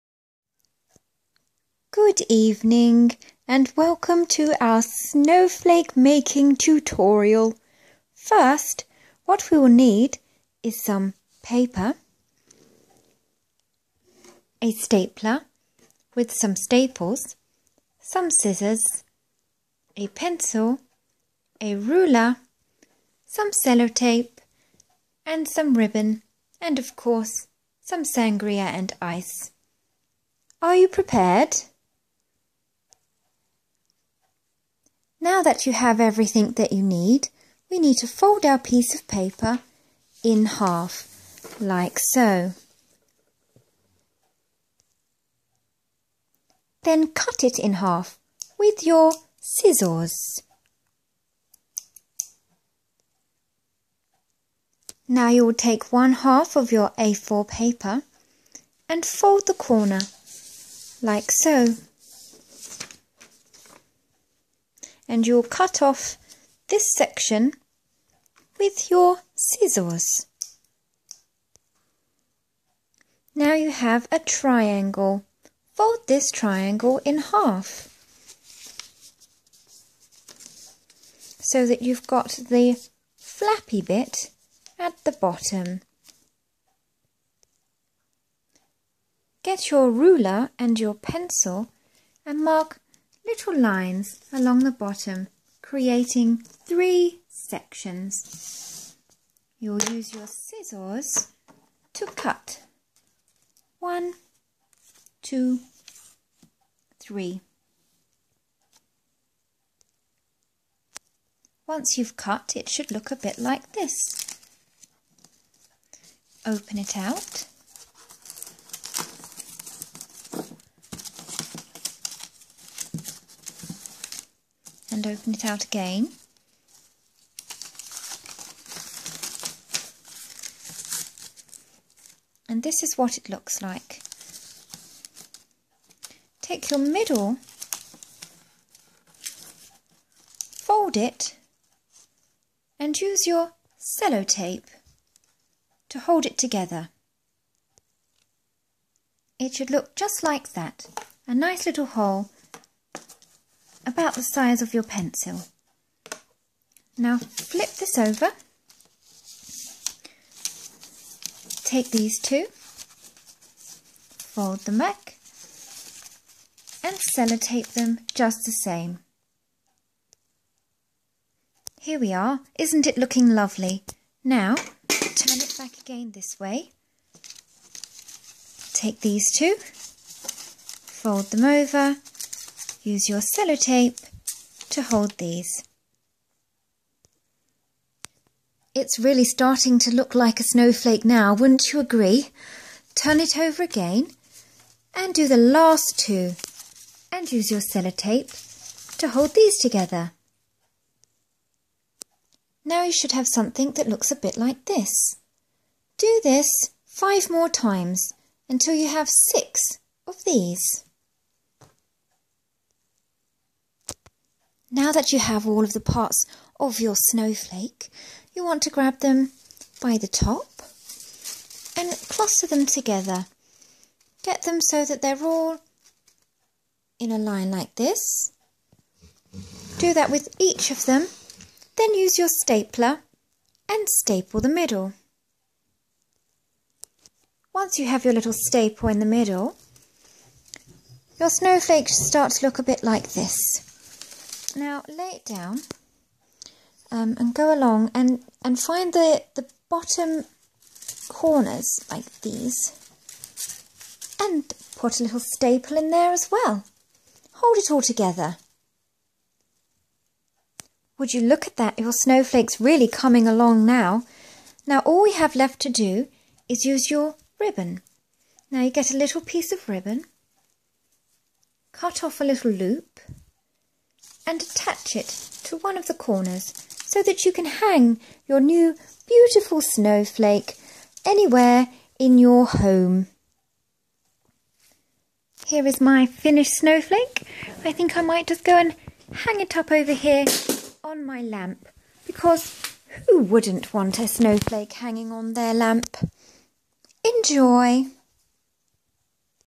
Making a paper snowflake
This is a quick tutorial on making a paper snowflake. good luck!